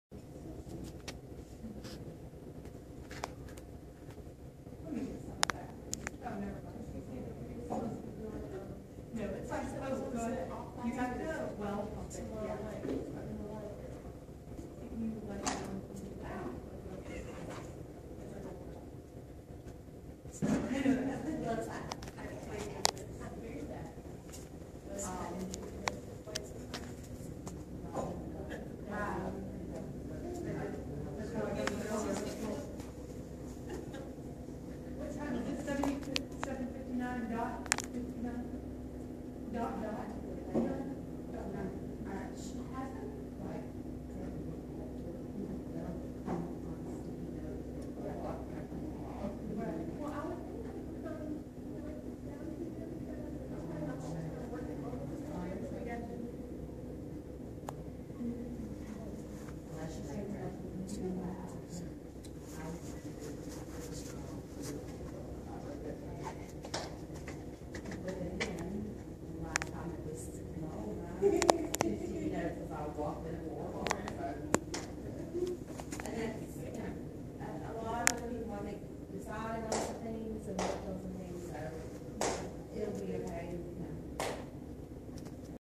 STAR WARS Ultimate Collector's Set Part 6
STAR WARS Ultimate Collector's Set is on display at MGCCC Library in Gautier, MS.